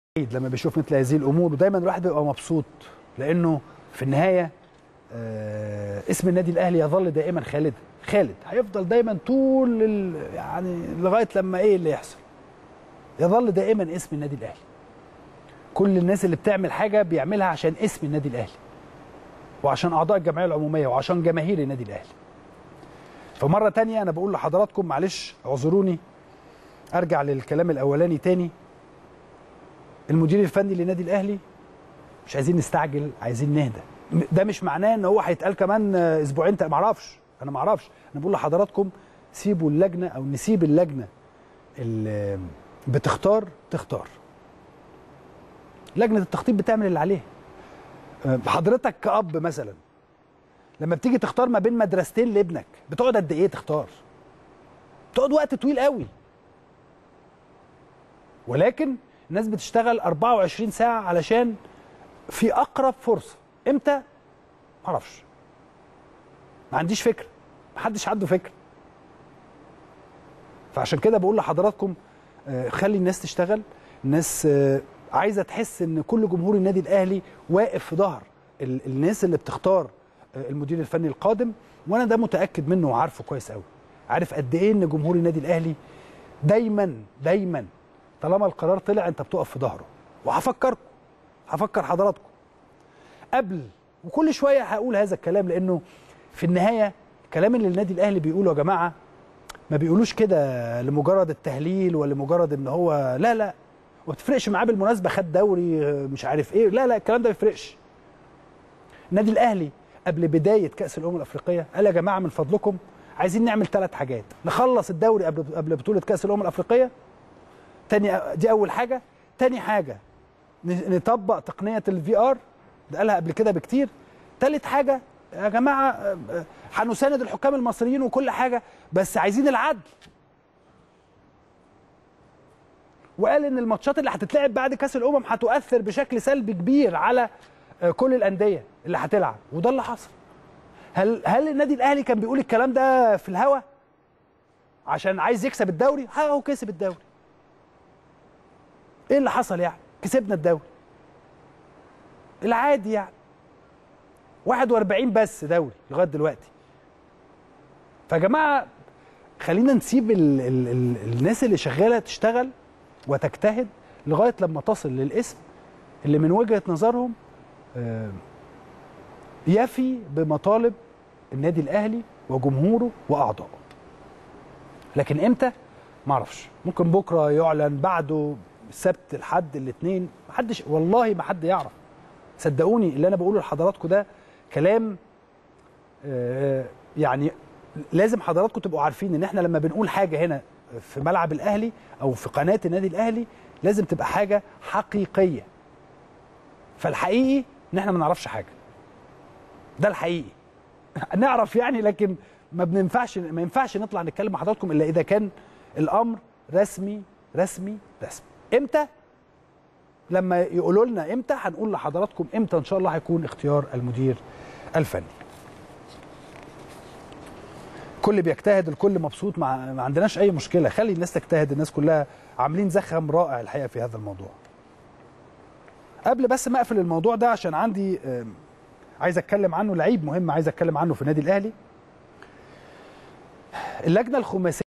اسلام الشاطر : لجنة التخطيط تجهد وتعمل 24 ساعة من أجل التعاقد مع مدرب يليق بقيمة وقدر النادى الاهلى
التردد: 12341 أفقي.
القمر: نايل سات.
معامل الخطأ: 5/6.
معدل الترميز: 27500